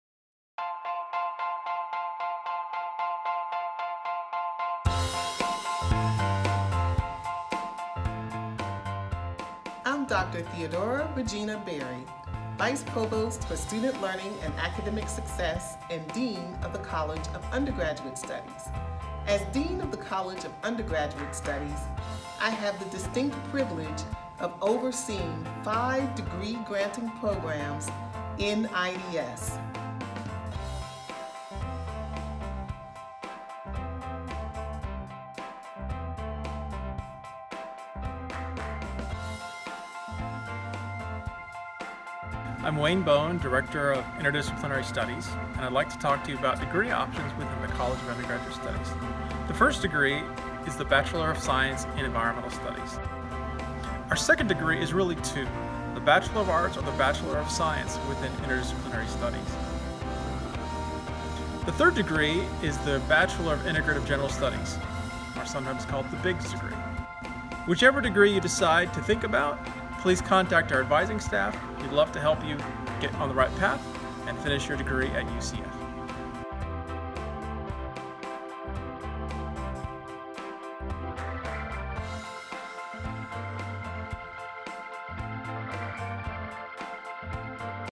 UCF College of Undergraduate Studies: Virtual Open House Presentation
The College of Undergraduate Studies offers the ability to tailor your program of study to suit your individual academic and career goals. This flexibility lets you choose your own path in environmental studies, women’s studies or interdisciplinary studies. For students pursuing degrees in science, technology, engineering or mathematics, the college administers the formerly National Science Foundation-funded EXCEL program to increase success in the freshman and sophomore science and mathematics courses as well as facilitate experiential learning, undergraduate research and advising.